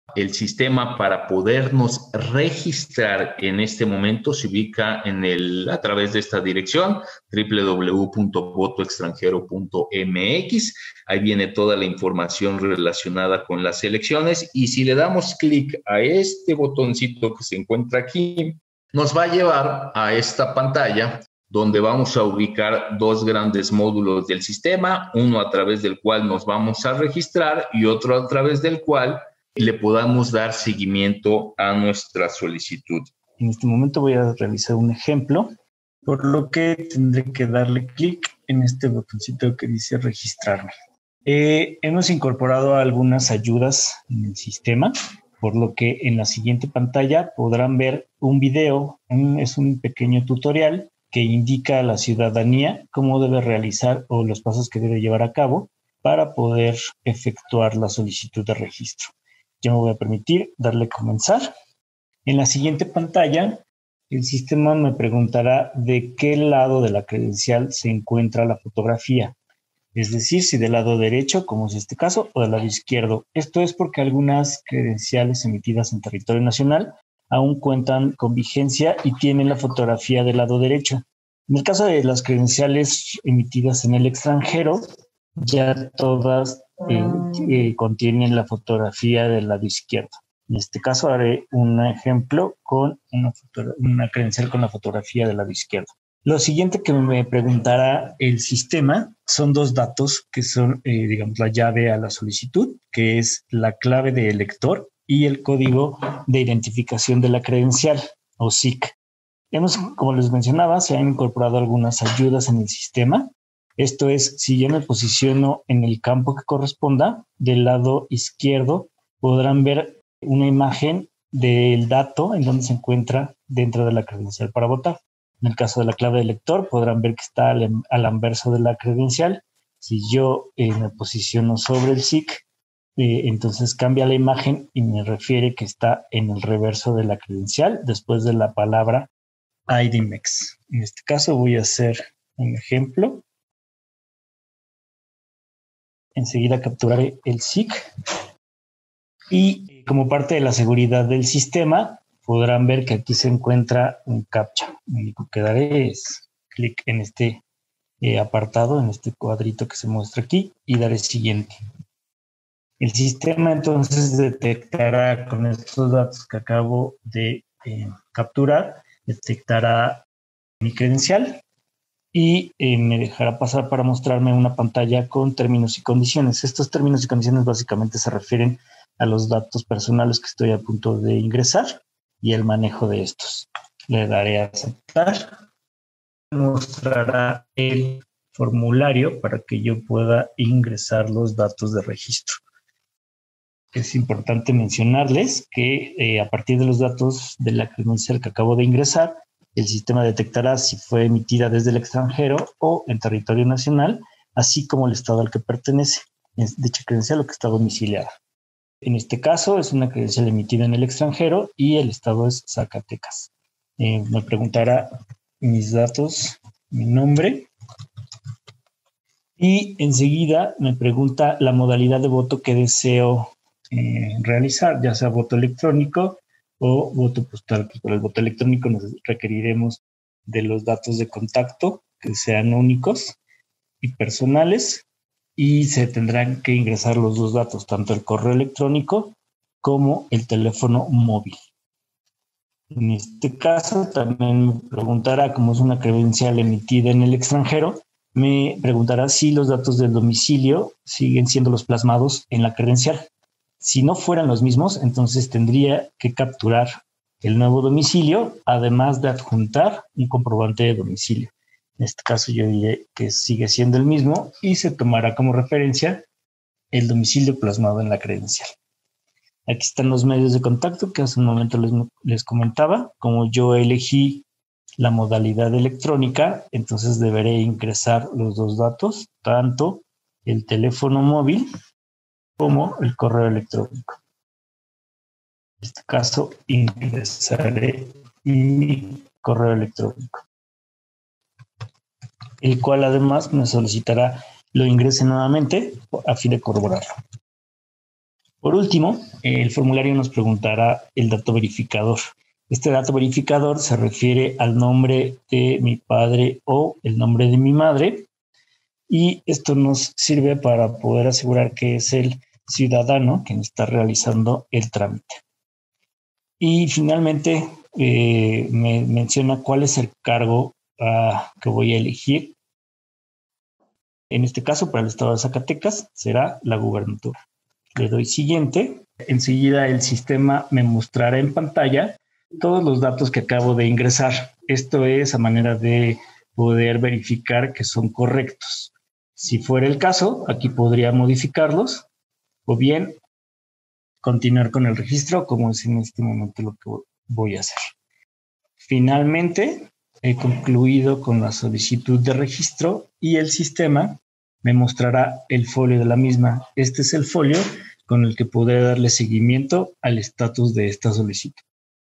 ¿Cómo me registro para votar desde el extranjero?
Para todas las y los zacatecanos que desean votar desde el extranjero, les  invitamos a ver el siguiente video en el que se explican detalladamente los pasos a seguir para registrarse y poder votar fuera del país.